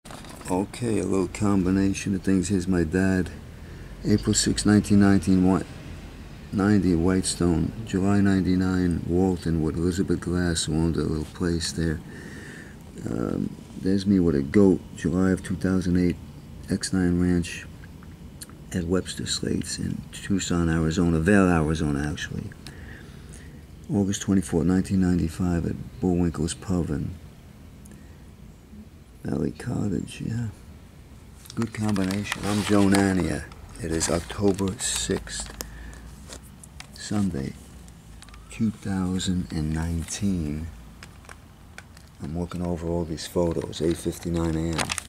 Joe Nania - dad whitestone new york
HERE ARE SOME PHOTOS OF MY         DAD FRANK NANIA  BACK  THEN ON FRIDAY APRIL 6 1990 - IN WHITESTONE , NEW YORK  - AND SOME MUSICIAN BAND MEMBERS - TOM GOLDEN  - TOMMY KOVACH  - 1995 - OF NYACK , NEW YORK